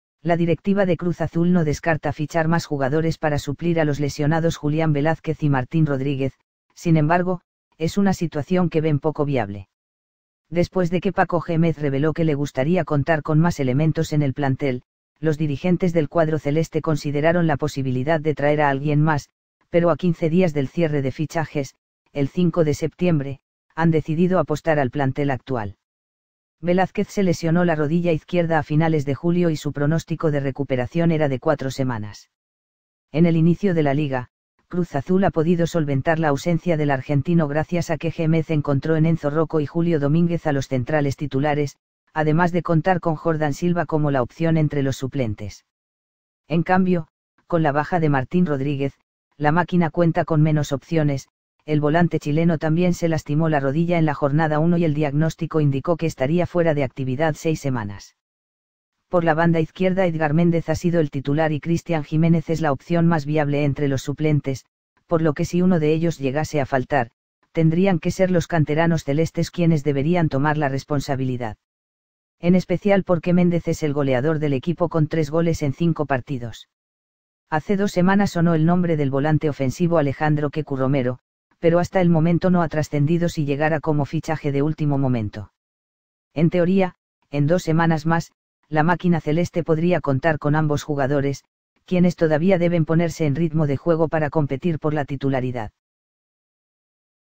Cruz Azul considera poco viable otro fichaje!
La directiva de Cruz Azul no descarta fichar más jugadores para suplir a los lesionados Julián Velázquez y Martín Rodríguez, sin embargo, es una situación que ven poco viable.

Después de que Paco Jémez reveló que le gustaría contar con más elementos en el plantel, los dirigentes del cuadro celeste consideraron la posibilidad de traer a alguien más, pero a 15 días del cierre de fichajes (el 5 de septiembre) han decidido apostar al plantel actual.

Velázquez se lesionó la rodilla izquierda a finales de julio y su pronóstico de recuperación era de cuatro semanas.
En el inicio de la Liga, Cruz Azul ha podido solventar la ausencia del argentino gracias a que Jémez encontró en Enzo Roco y Julio Domínguez a los centrales titulares, además de contar con Jordan Silva como la opción entre los suplentes.
En cambio, con la baja de Martín Rodríguez, la Máquina cuenta con menos opciones, el volante chileno también se lastimó la rodilla en la Jornada 1 y el diagnóstico indicó que estaría fuera de actividad seis semanas.

Por la banda izquierda Édgar Méndez ha sido el titular y Christian Giménez es la opción más viable entre los suplentes, por lo que si uno de ellos llegase a faltar, tendrían que ser los canteranos celestes quienes deberían tomar la responsabilidad.
En especial porque Méndez es el goleador del equipo con tres goles en cinco partidos.

Hace dos semanas sonó el nombre del volante ofensivo Alejandro “Keku” Romero, pero hasta el momento no ha trascendido si llegara como fichaje de último momento.

En teoría, en dos semanas más, la Máquina celeste podría contar con ambos jugadores, quienes todavía deben ponerse en ritmo de juego para competir por la titularidad.